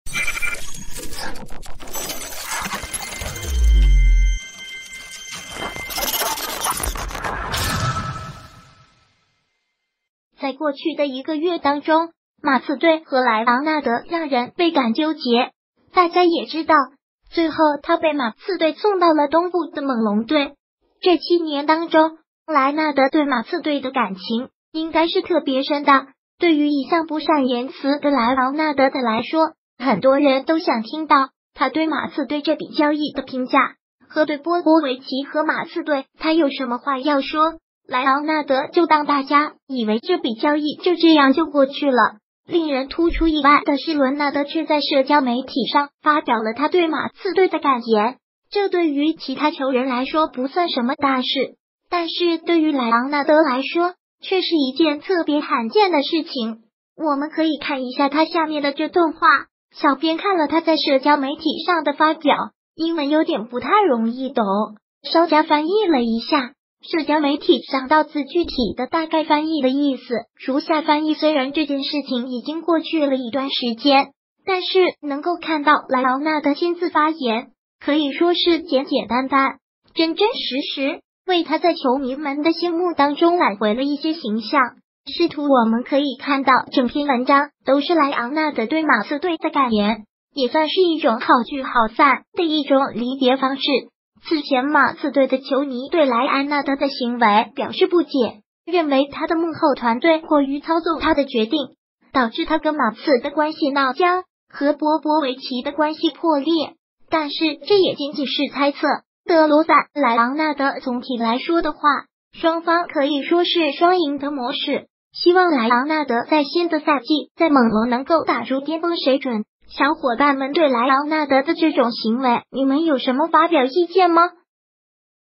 前马刺巨星莱昂纳德终于发声了！不善言辞的他让人出乎意料
在过去的一个月当中，马刺队和莱昂纳德让人倍感纠结，大家也知道，最后他被马刺队送到了东部的猛龙队。这七年当中，莱纳德对马刺队的感情应该是特别深的，对于一向不善言辞的莱昂纳德的来说，很多人都想听到他对马刺队这笔交易的评价，和对波波维奇和马刺队他有什么话要说？莱昂纳德就当大家以为这笔交易就这样就过去了，令人突出意外的是伦纳德却在社交媒体上发表了他对马刺队的感言。这对于其他球员来说不算什么大事，但是对于莱昂纳德来说却是一件特别罕见的事情。我们可以看一下他下面的这段话，小编看了他在社交媒体上的发表，英文有点不太容易懂，稍加翻译了一下。社交媒体上到字具体的大概翻译的意思如下翻译虽然这件事情已经过去了一段时间，但是能够看到莱昂纳德亲自发言，可以说是简简单单，真真实实，为他在球迷们的心目当中挽回了一些形象。师徒我们可以看到整篇文章都是莱昂纳德对马刺队的感言，也算是一种好聚好散的一种离别方式，此前马刺队的球迷对莱安纳德的行为表示不解，认为他的幕后团队过于操纵他的决定，导致他跟马刺的关系闹僵，和波波维奇的关系破裂，但是这也仅仅是猜测。德罗赞莱昂纳德总体来说的话，双方可以说是双赢的模式，希望莱昂纳德在新的赛